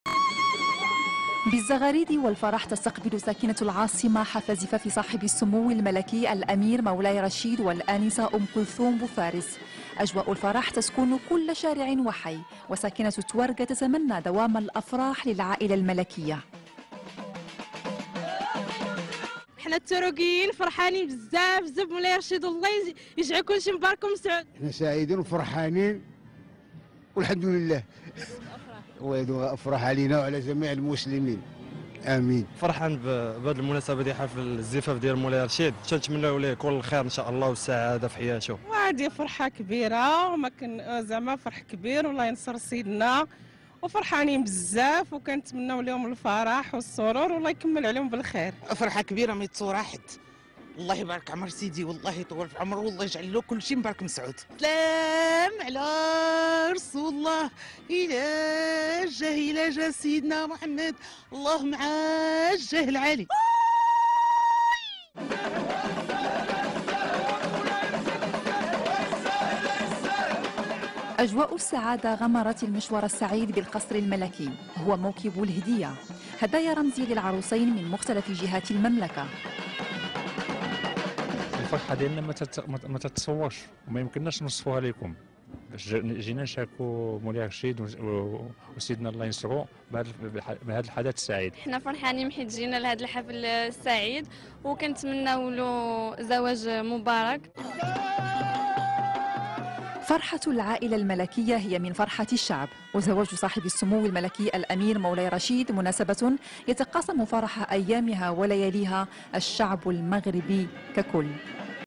حفل زفاف صاحب السمو الملكي الأمير مولاي رشيد: تهاني المواطنين
المواطنون من مختلف مدن المملكة يقدمون التهاني للأسرة الملكية